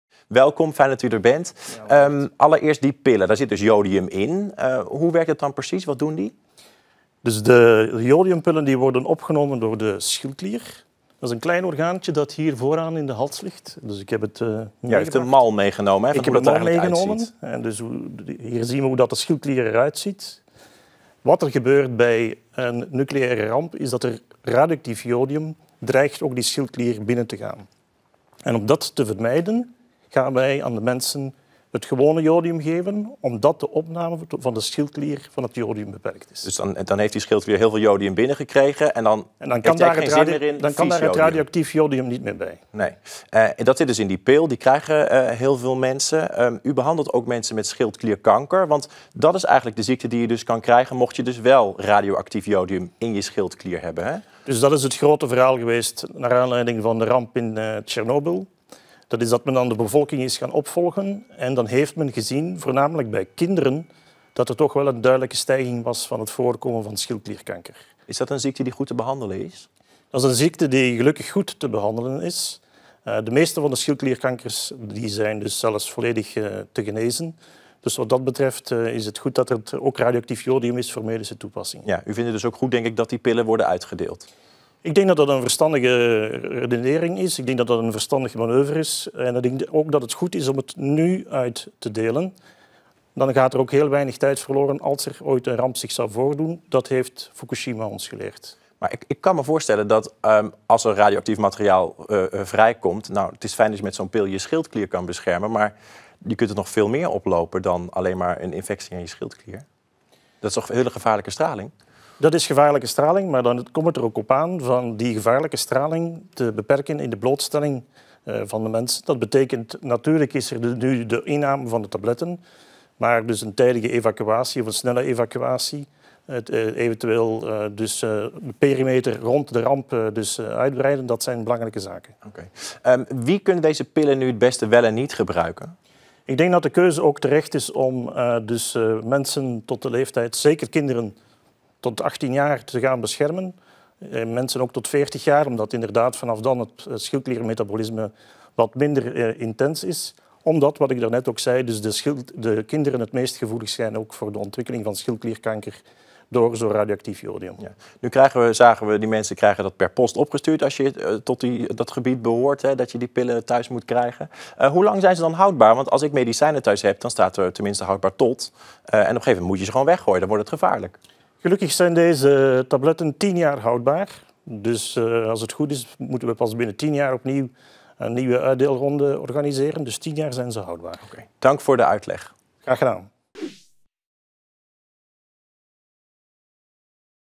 Hoe werken jodiumpillen? Nucleair geneeskundige Wyndaele geeft uitleg.
Honderdduizenden Brabantse huishoudens ontvangen in oktober 2017 jodiumtabletten van het Ministerie van Volksgezondheid. Die zijn bedoeld om de bevolking te beschermen in het geval van een nucleaire ramp. Voorkomen is beter dan genezen, redeneert de overheid.

Met kerncentrales in het Zeeuwse Borsele, de Belgische plaatsen Doel en Tihange en onderzoekscentrum Euratom Mol, onder de rook van Eindhoven, behoort Brabant tot de risicogebieden waar radioactieve neerslag terecht kan komen.

Wat die met de bevolking kan doen, is duidelijk sinds de ramp met de Oekraïense kerncentrale in Tsjernobyl. Daar sterven meer mensen aan schildklierkanker als gevolg van radioactief jodium dat vrijkwam.

Wie binnen acht uur na een kernramp jodiumtabletten slikt, kan zich tegen dit type kanker wapenen.

Het menselijk lichaam slaat (tijdelijk) jodium uit de tabletten op in de schildklier. De schildklier regelt de stofwisseling in het lichaam. Als radioactief jodium wordt opgeslagen in de schildklier, kan het op termijn zorgen voor schildklierkanker. Het preventief slikken van jodiumtabletten zorgt ervoor dat de schildklier tijdelijk verzadigd wordt met stabiel jodium. Hierdoor wordt het radioactieve jodium dat mensen binnenkrijgen niet opgeslagen in de schildklier, maar juist uitgescheiden.

Bron: Omroep Brabant